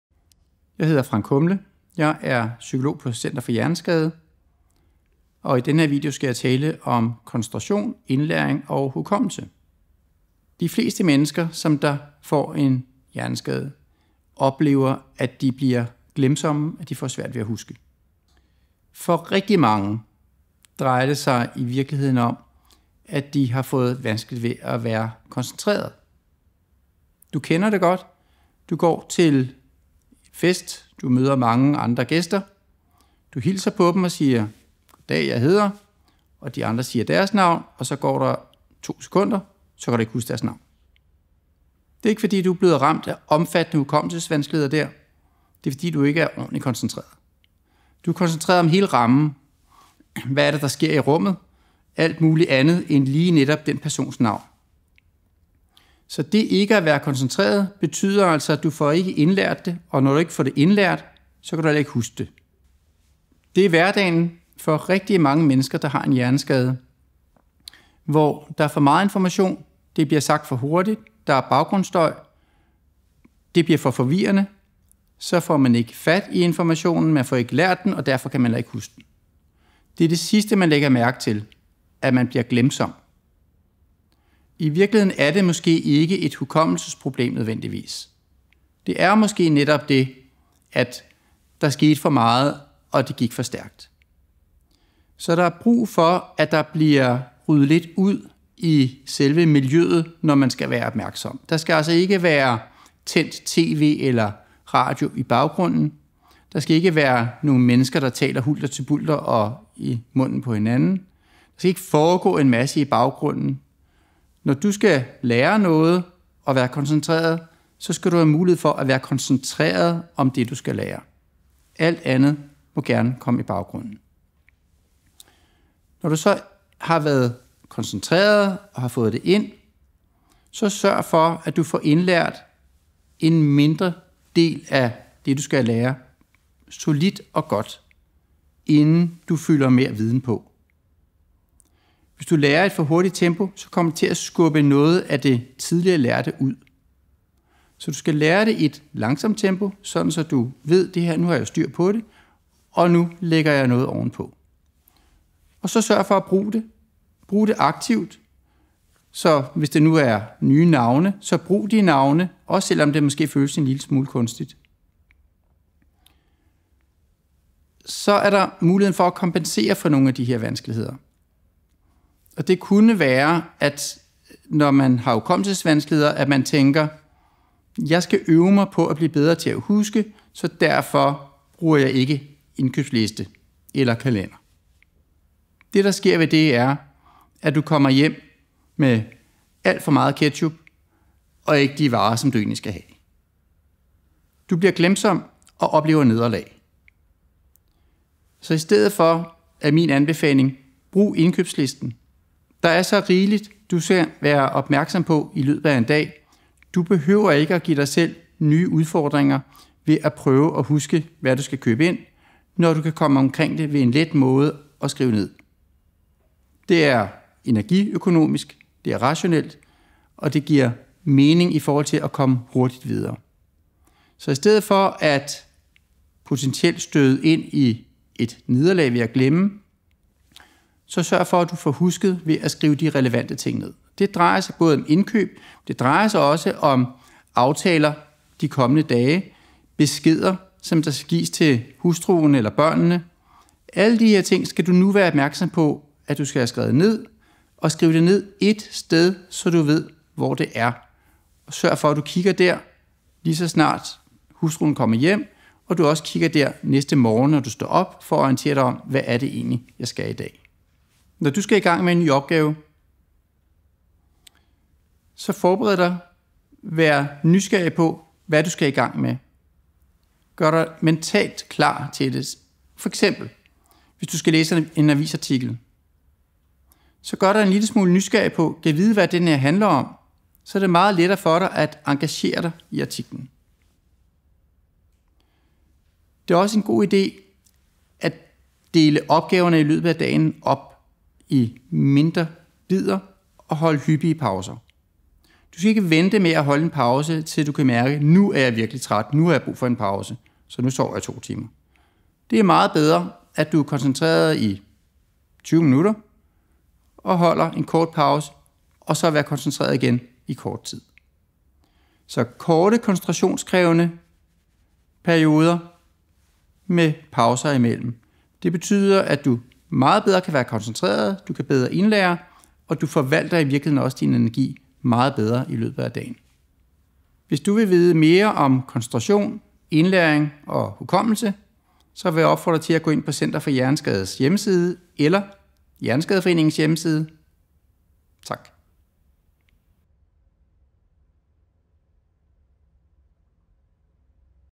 Koncentration, indlæring og hukommelse
Mange oplever efter en hjerneskade at de har fået svært ved at huske. Men er det i virkeligheden hukommelsen den er gal med?

Psykolog Frank Humle fra Center for Hjerneskade fortæller i denne video om koncentration, indlæring og hukommelse og giver råd til hvordan man kan træne sig bedre til at huske.

Filmene er produceret af Georg Works.